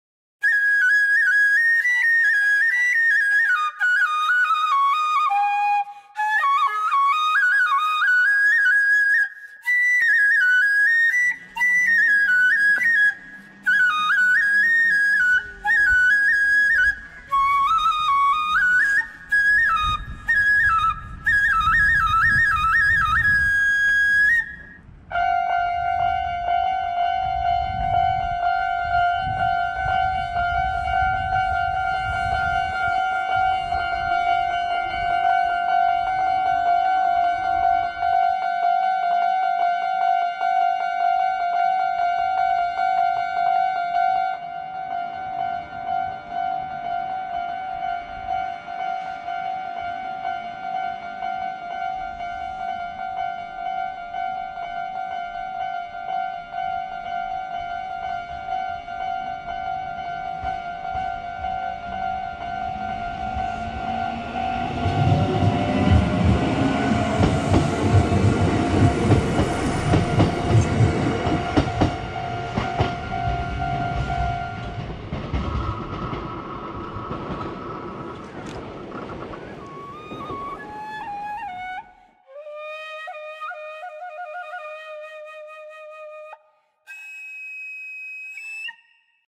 【Japanese Train】 Kintetsu Shinjo. The train to pass at the railroad crossing
Google map for this video

◎Cameras
・SONY a7iii
・DJI